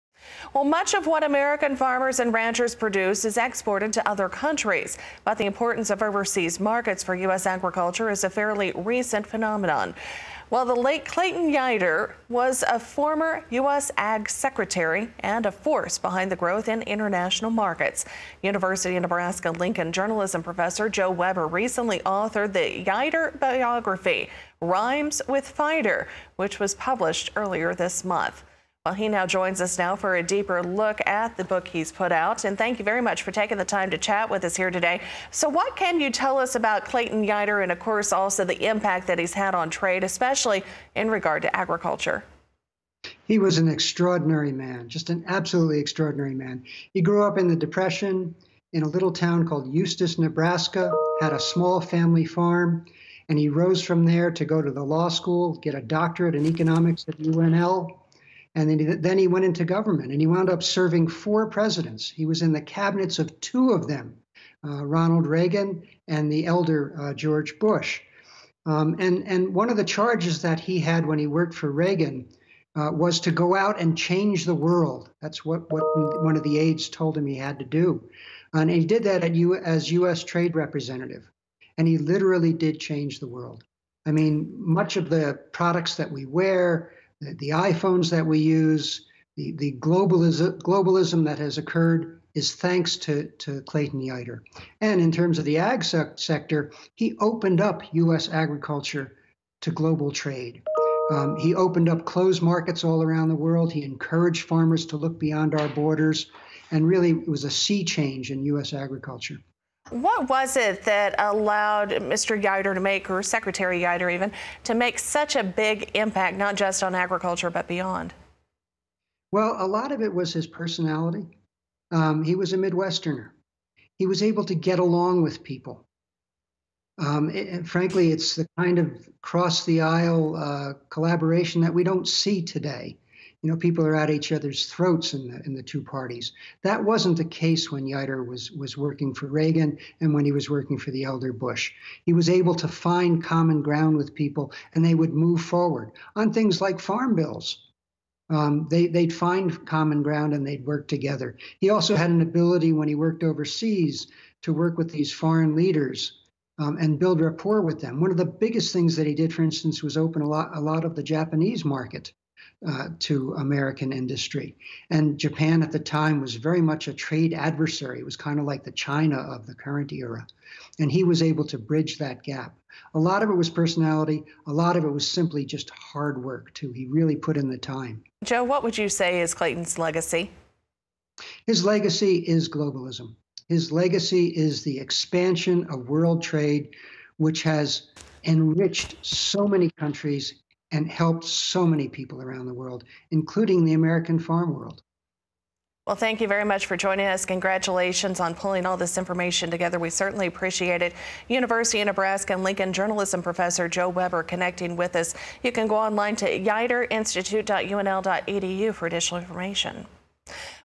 On RFD-TV: Joseph Weber
University of Nebraska-Lincoln journalism professor Joe Weber talks live about his new biography of international trade pioneer Clayton Yeutter. Aired December 7, 2021 on RFD-TV.